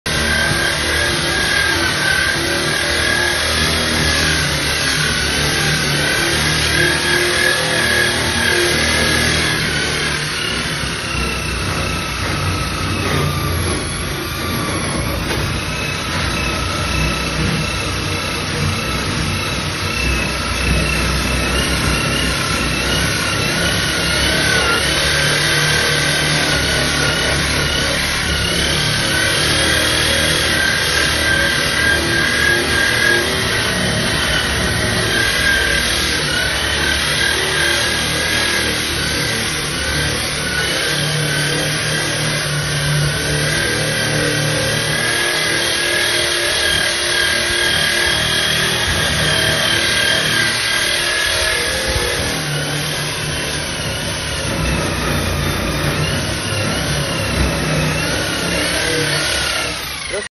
1 MINUT A TIVISSA 3 5 2024
1 Minut a Tivissa. Avui el Vicens Borràs Cedó de Ca Rovira, m'ha fet lo minut. 08:56 h del 3-5-2024